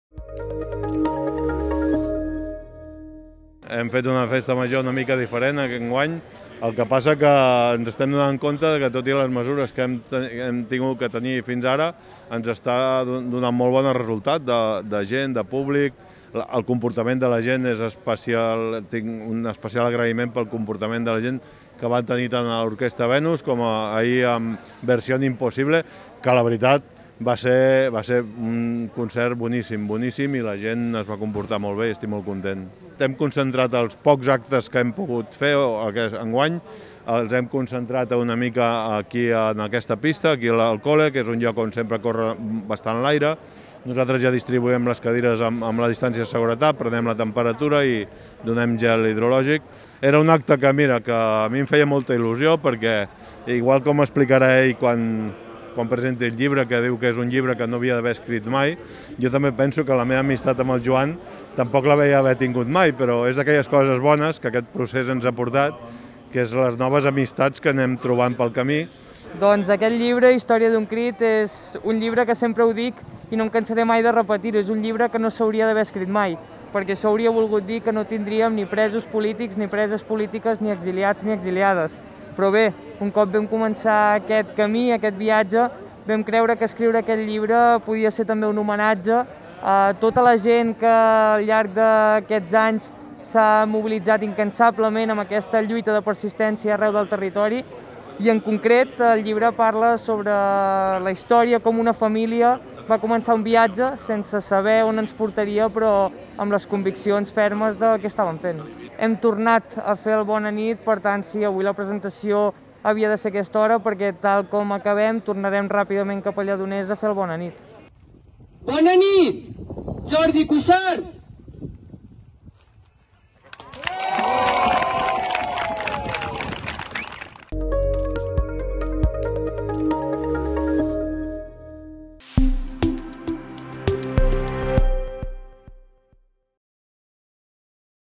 "Història d'un crit"
Presentació del llibre "Història d'un crit" de Joan BonaNit, ens ho explica Joan Porras dins els actes de la Festa Major de Balenyà